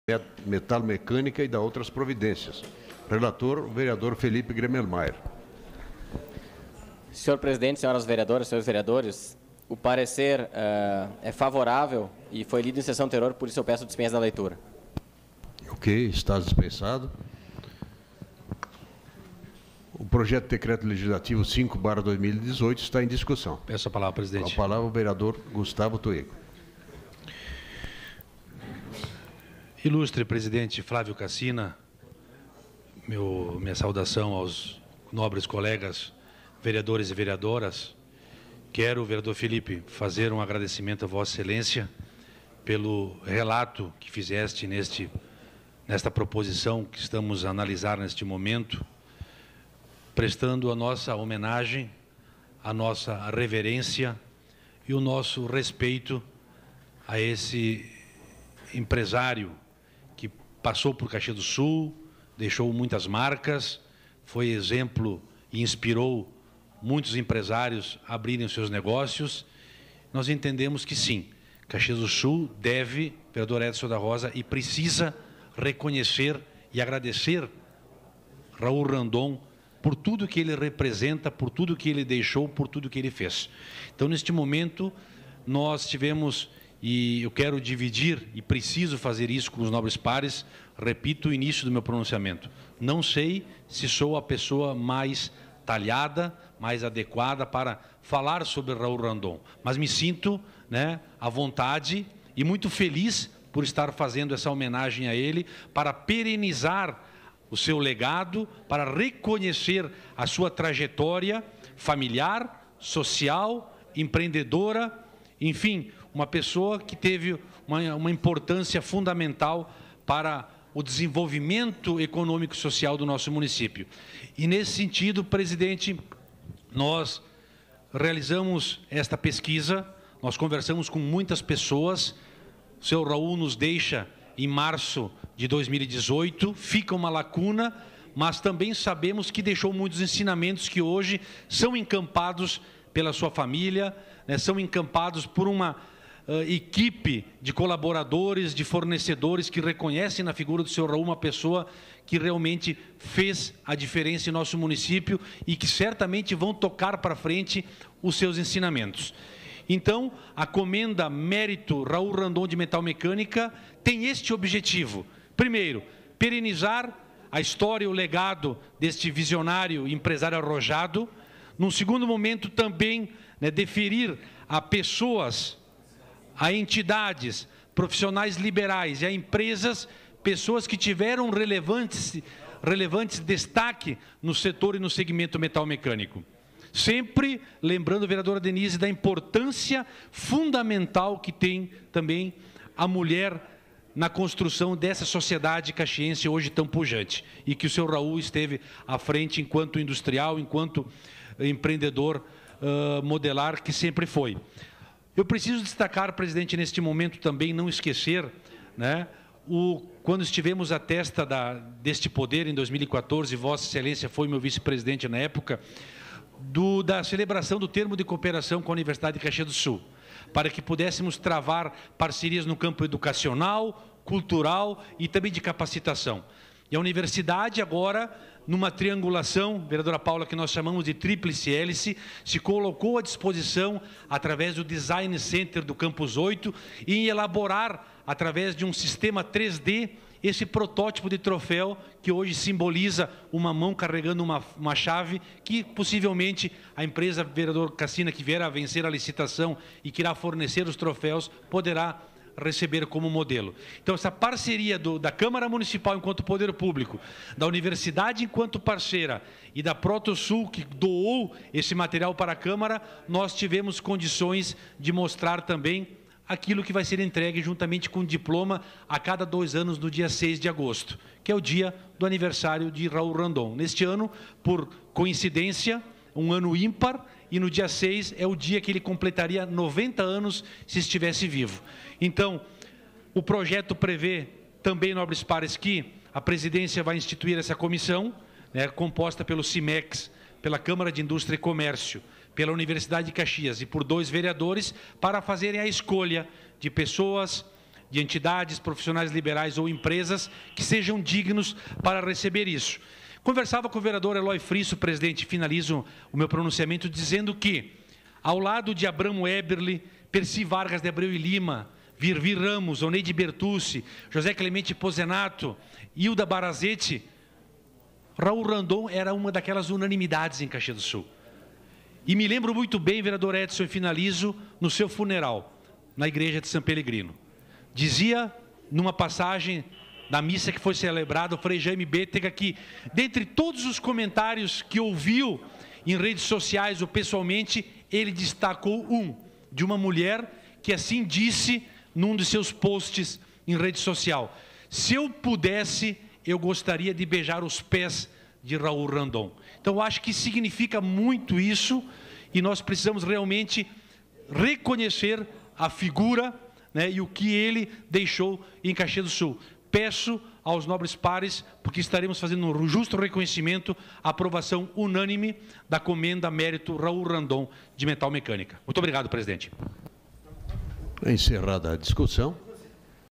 18 Ordem do Dia - Segunda Discussão do PDL 5/2018
Manifestação do vereador Gustavo Toigo sobre o Projeto de Decreto Legislativo que institui a Comenda Mérito Raul Randon de Metalmecânica, de sua autoria.